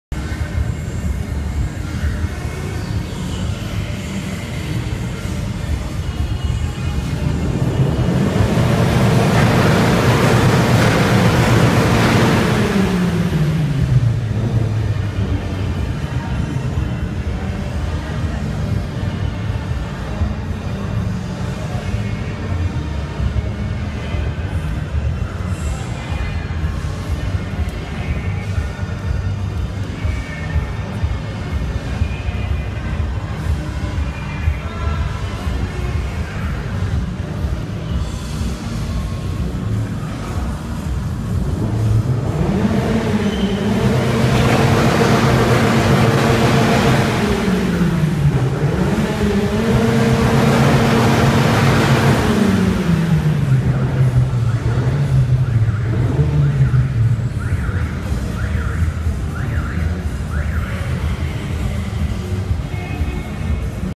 Lowville 2016
Короткие моменты выставки L.A.W. 2016 от Wheels Boutique Ukraine